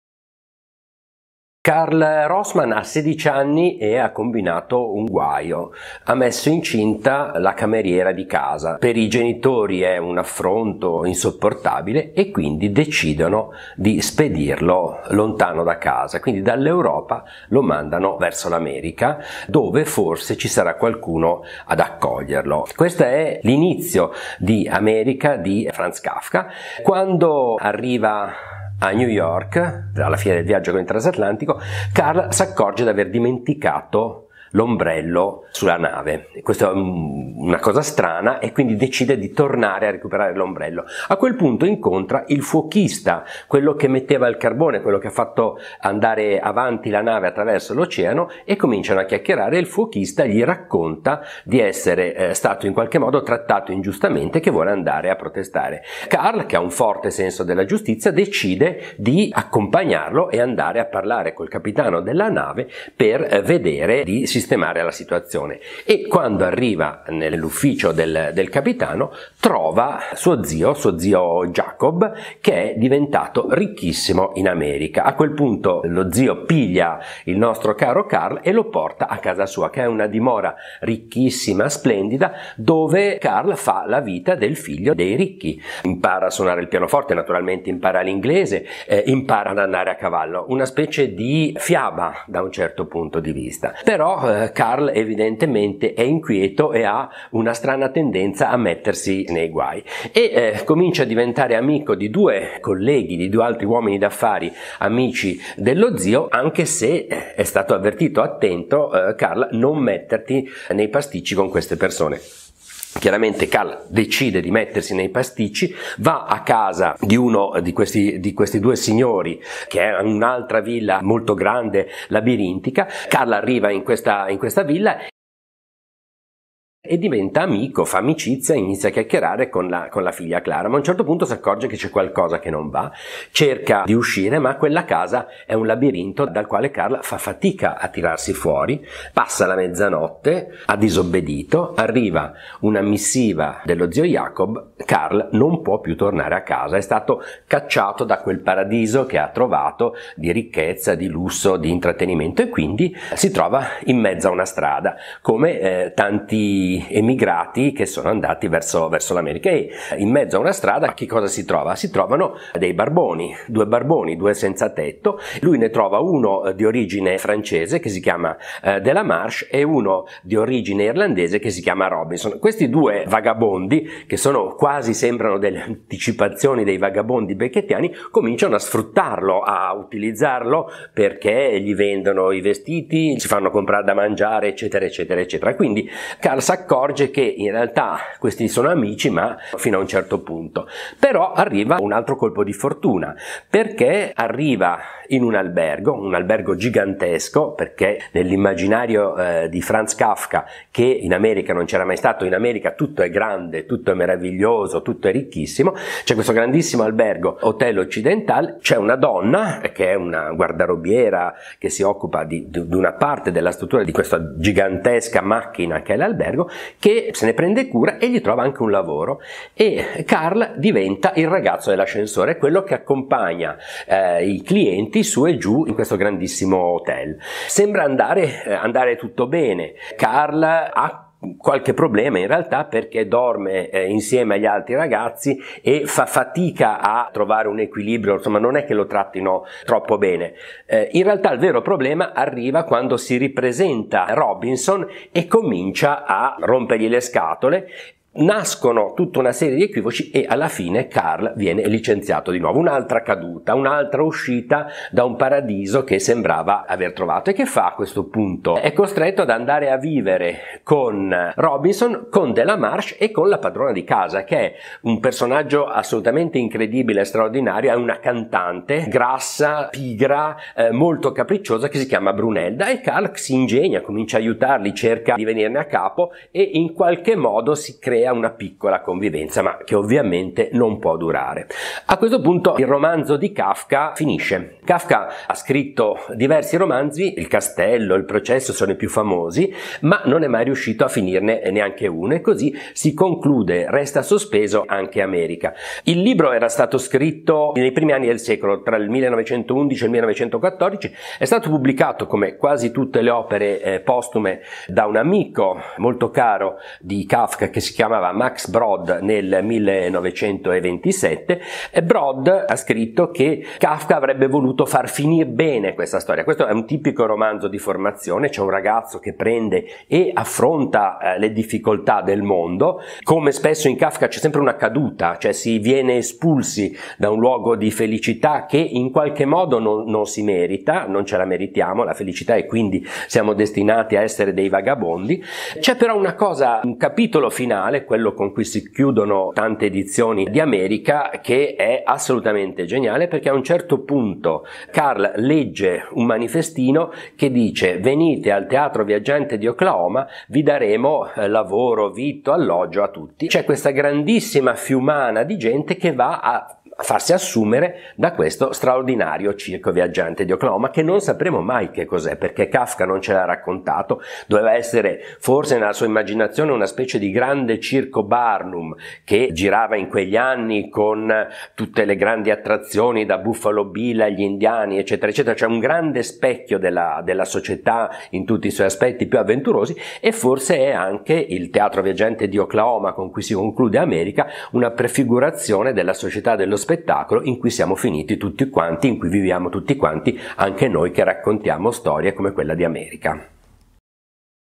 America, di Franz Kafka, raccontato e spiegato da Oliviero Ponte di Pino
Una storia di formazione dall'Europa alla scintillante e ricca America, un viaggio faticoso, pieno di colpi di fortuna e di cadute rovinose.
Un racconto di Oliviero Ponte di Pino